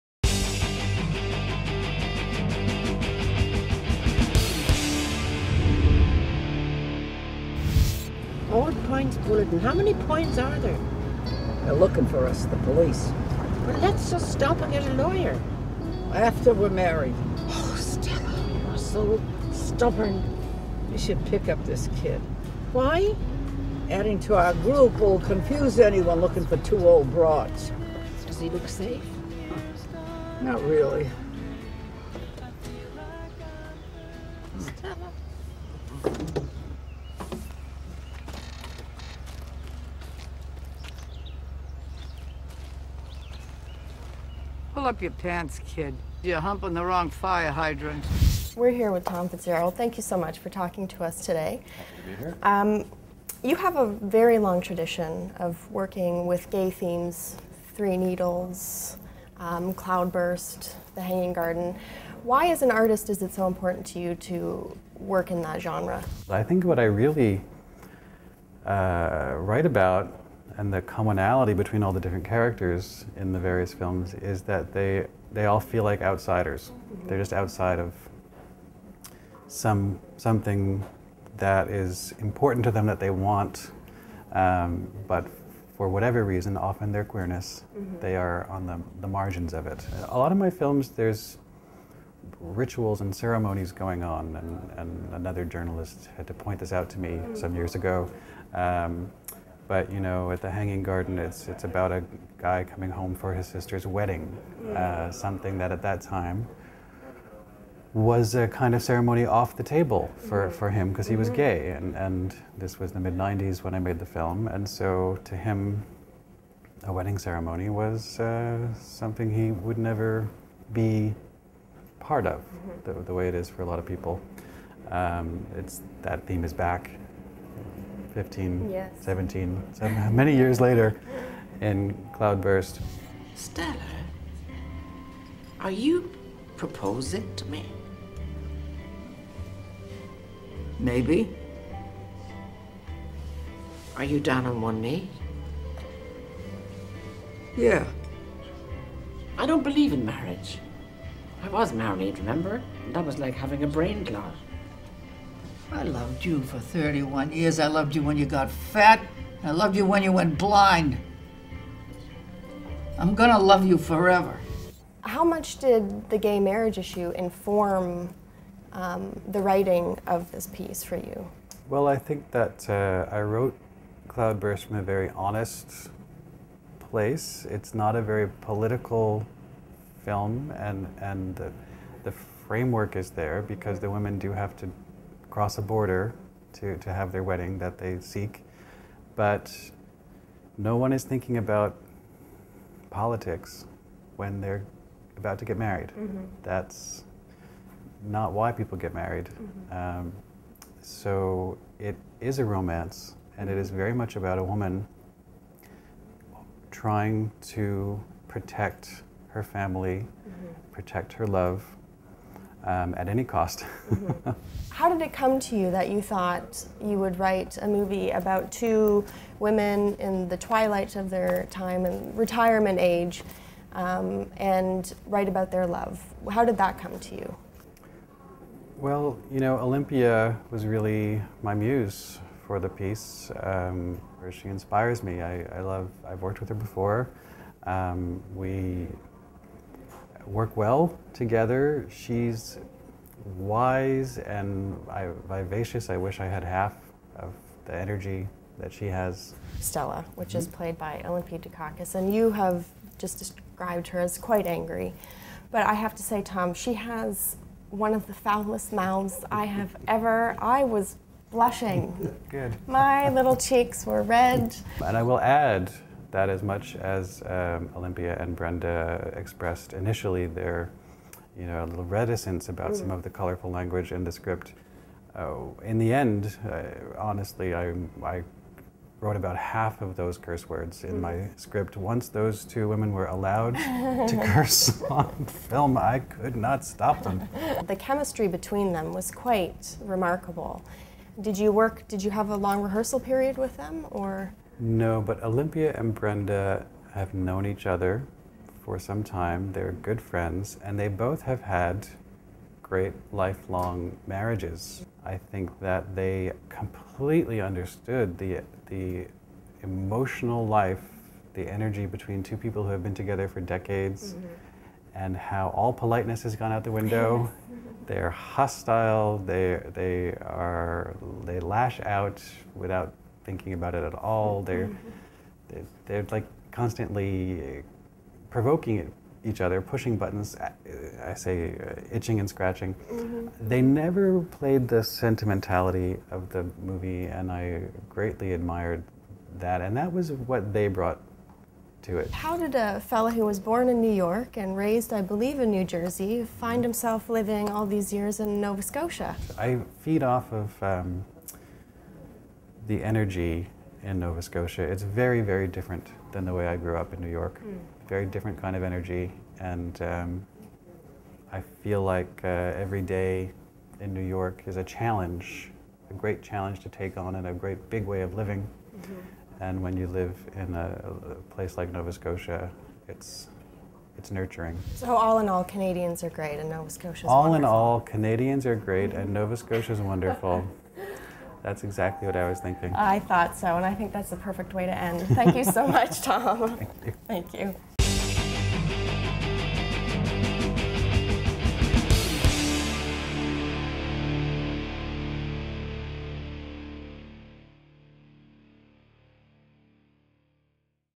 Thom Fitzgerald Cloudburst Interview - Queeriesmag
Renée Olbert  interviews acclaimed Canadian director Thom Fitzgerald about his Lesbian road-trip film, Cloudburst.

Queeriesmag.com - For queer Canadian women and other lovely like-minded folks.